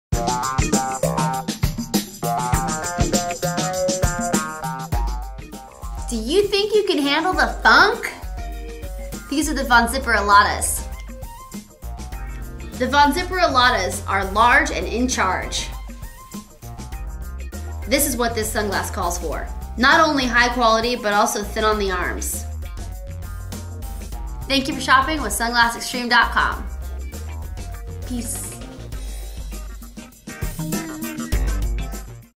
VonZipper Alotta Sunglasses
VonZipper Alotta sunglasses available at SunglassExtreme.com.

* 6 Base Spherical Lens
* Stainless Steel Optical Hinges
* Durable, high-quality material
* 100 Percent UV Protection